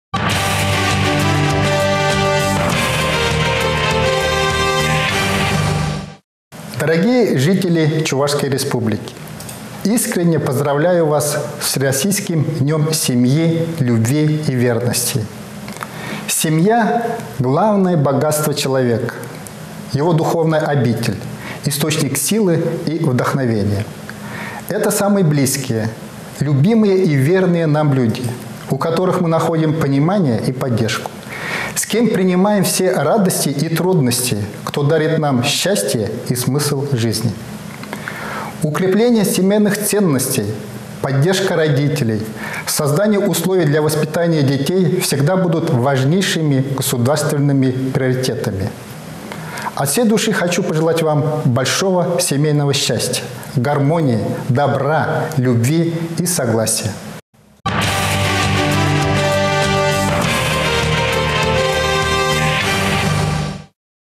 Поздравление врио Главы Чувашии Олега Николаева с Днем семьи, любви и верности
Поздравление временнно исполняющего обязанности Главы Чувашской Республики Олега Николаева с Днем семьи, любви и верности